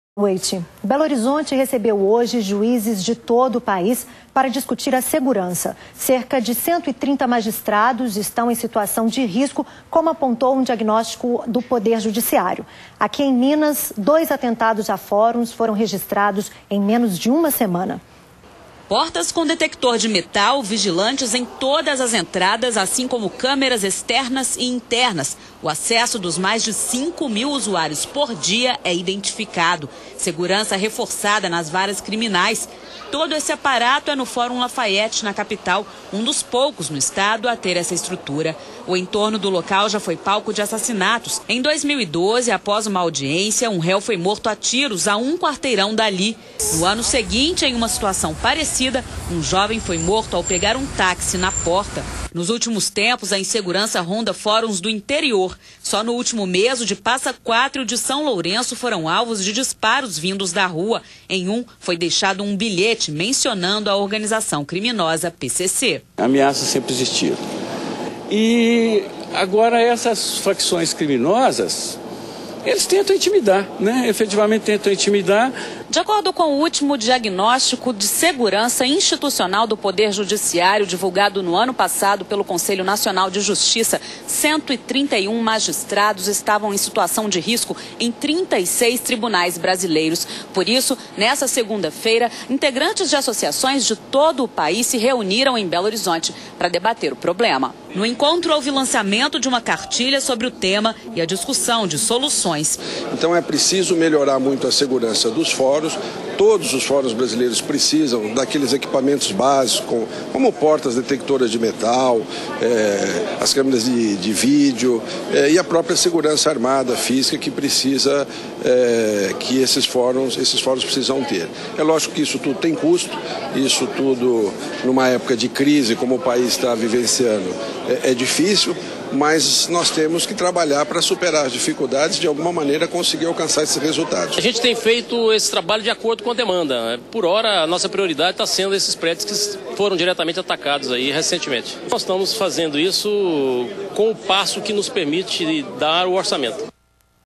Segurança em Fóruns é discutida em BH
Juízes do Brasil inteiro se reuniram em Belo Horizonte para discutir sobre a segurança e debater possíveis soluções. Só no Estado, dois atentados foram registrados em menos de uma semana.